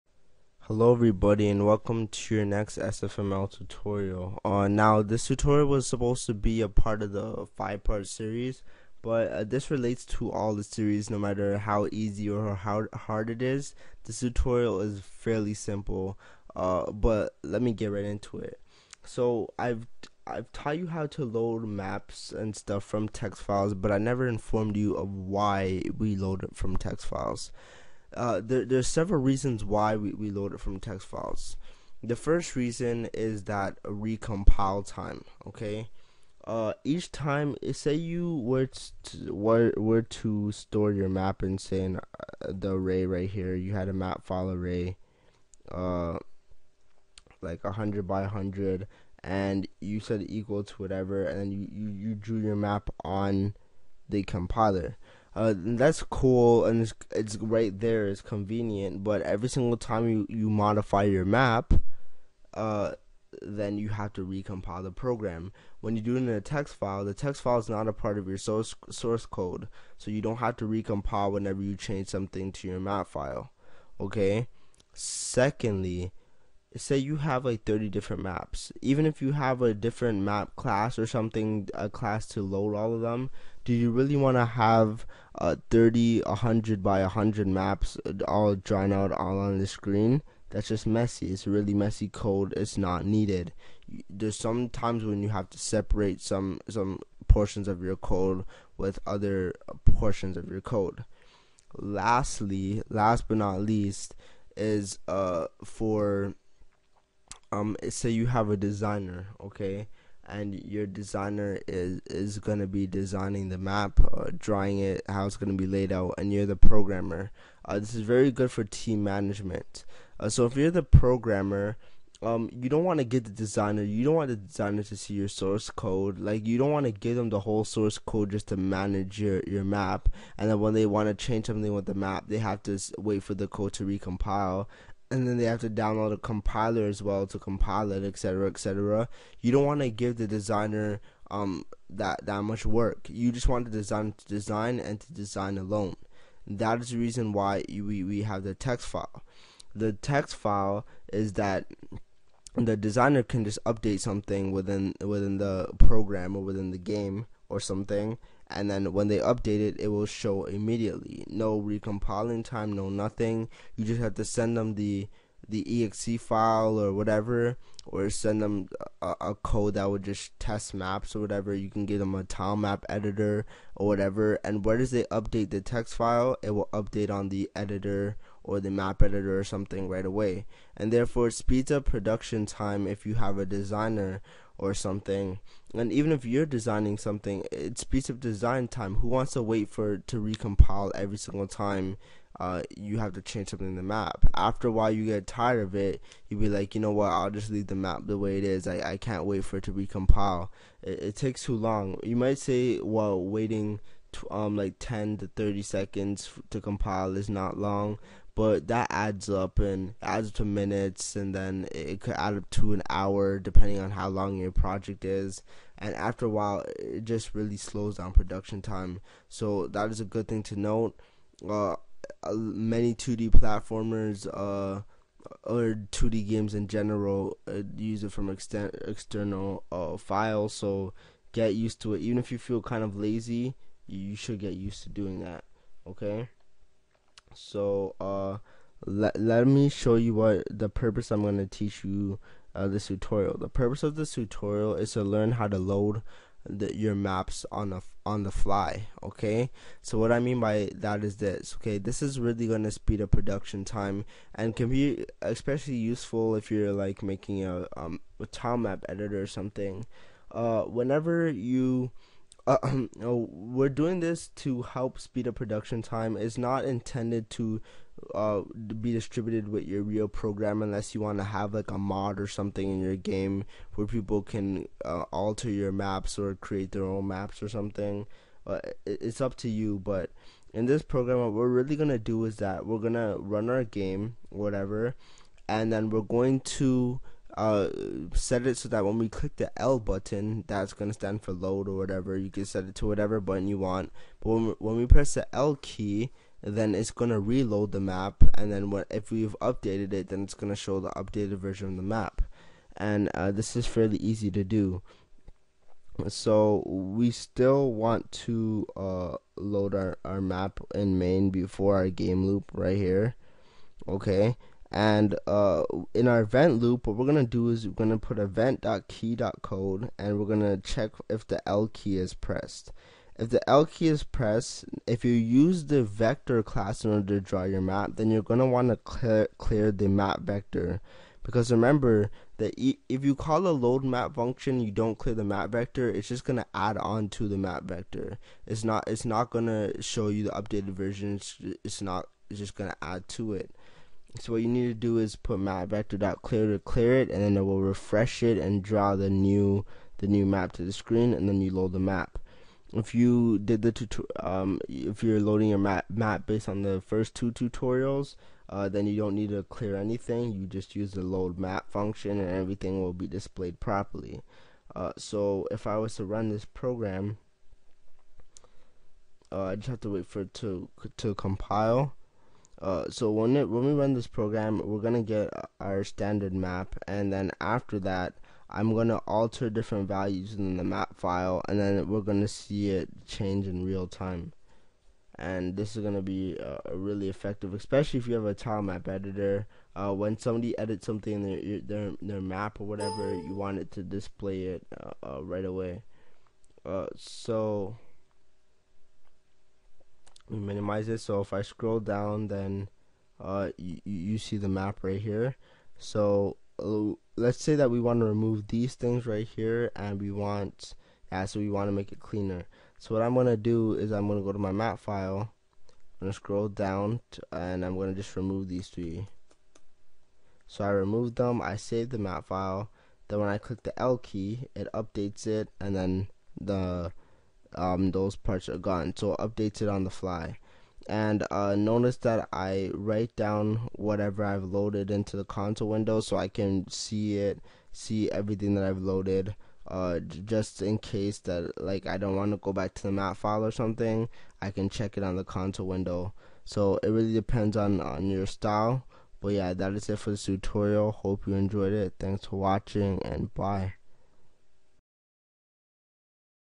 C++ Sfml 1.6 Made Easy Tutorial 33 - Loading Maps in Real Time
This tutorial will teach you on how to load maps on the go(in real time). This can help when you're designing tile map editors or something of that manner. It can also be used to help speed up production time. So I hope you enjoy :)